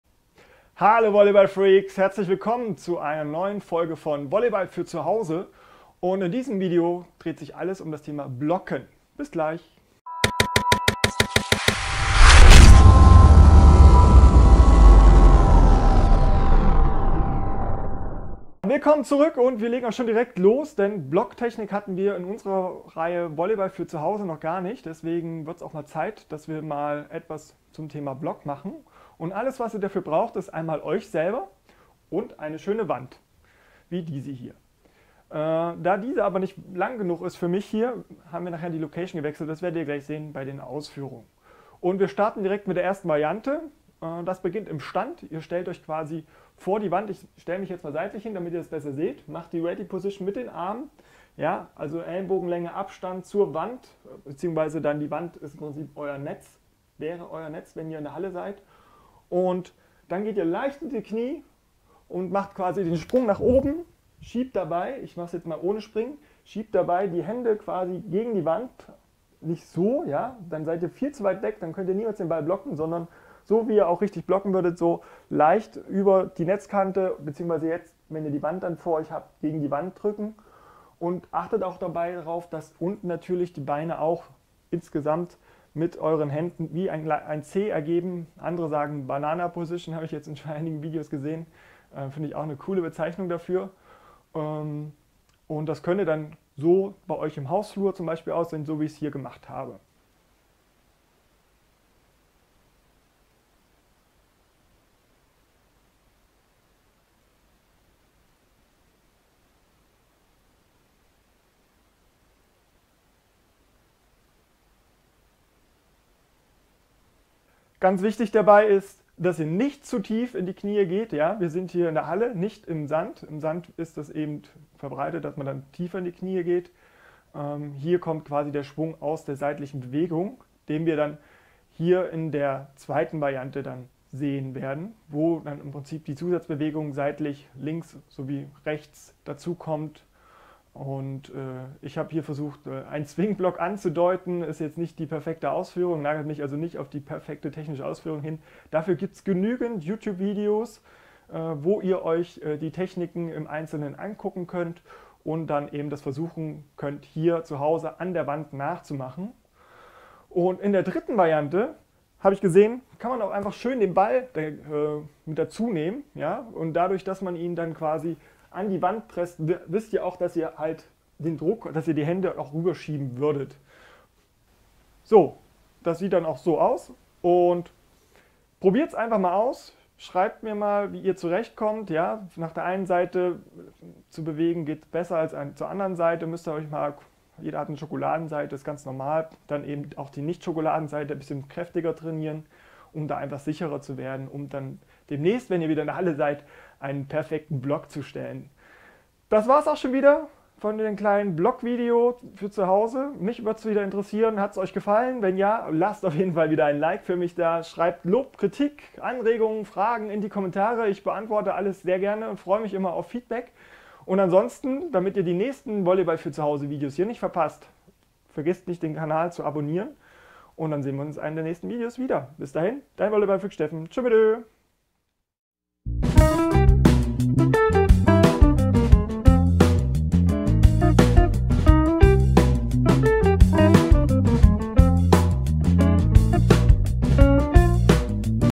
Volleyball für zu Hause - Übung 11: Blocken an der Wand
Im Format "Volleyball für zu Hause" zeigt dir Volleyballfreak kleine Übungen zur Technik, Fitness und mehr, damit du auch daheim nicht auf Volleyball verzichten musst und dich selber weiter voran bringen kannst.

Übung 11: Blocken an der Wand

1. Variante: Blocken aus dem Stand (achten auf richtiges C/Bananaposition)
2. Variante: Block aus der seitlichen Bewegung
3. Variante: Blocken mit Ball

Hier kommst du zu unserem Shop mit über 100 Volleyballmotiven für T-Shirts, Hoodies uvm.: